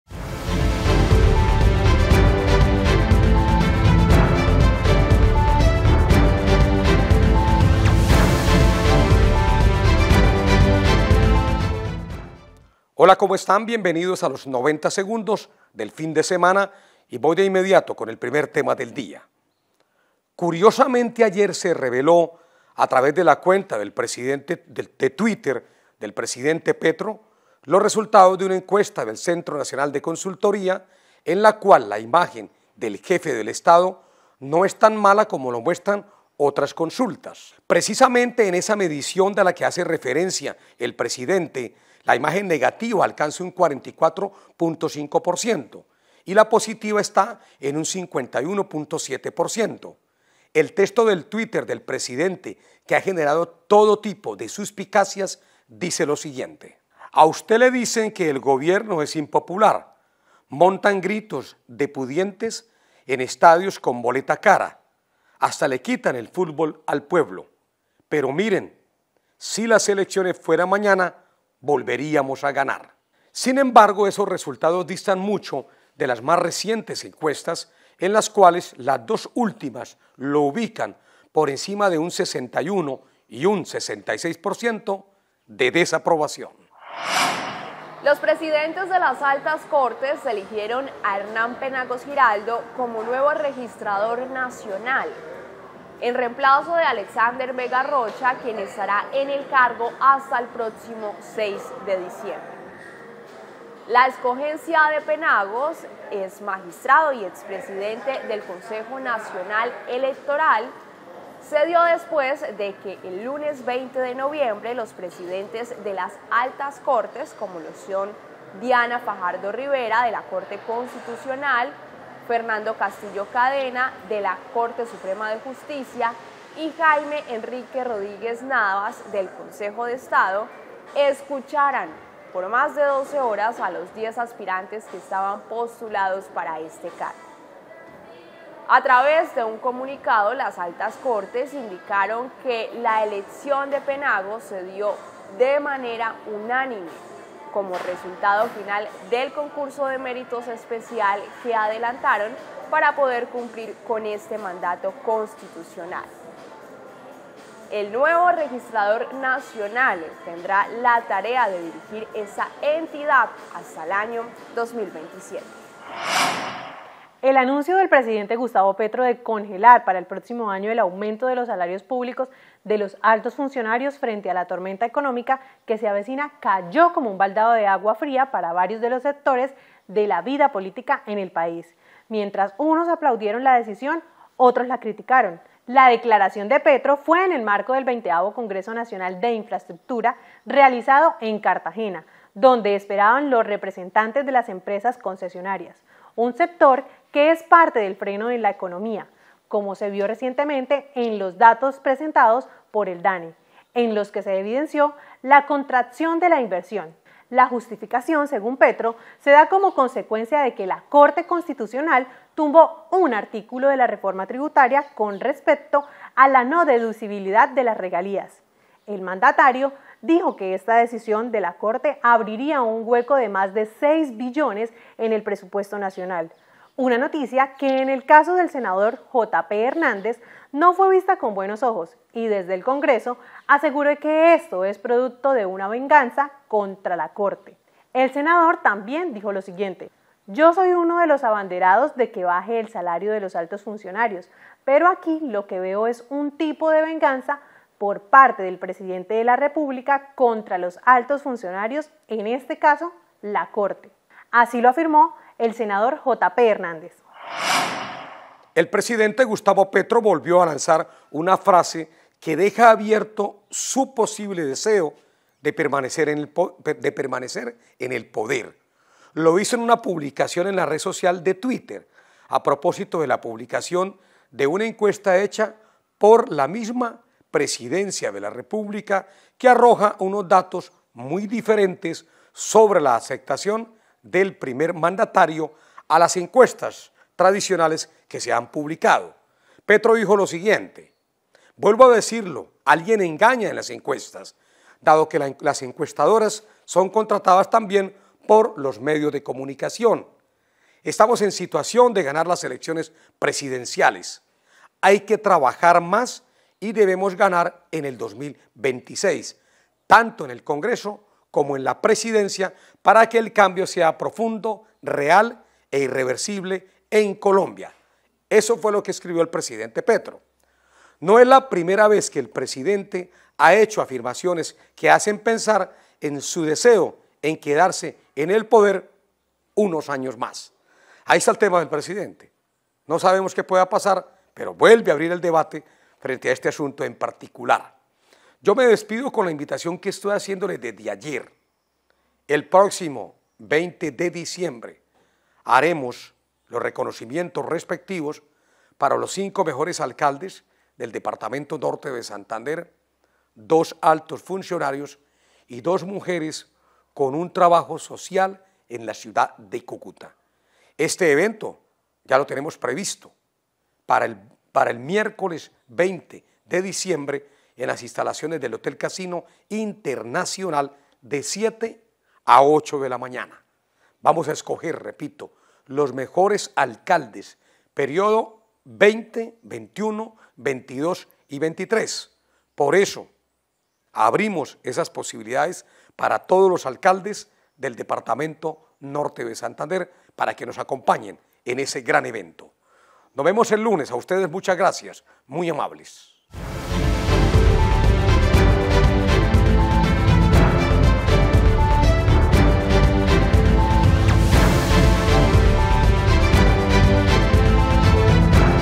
Encuesta del CNC muestra que la imagen de Petro no es tan mala
90Segundos Viernes 24 de Noviembre

Con Fredy Fernando Parada Temas:

1. Encuesta del CNC muestra que la imagen de Petro no es tan mala
2. Hernán Penagos Giraldo es el nuevo registrador Nacional
3. Jota Pe Hernández: “una venganza contra la corte”
4. Presidente Petro quiere quedarse en el poder?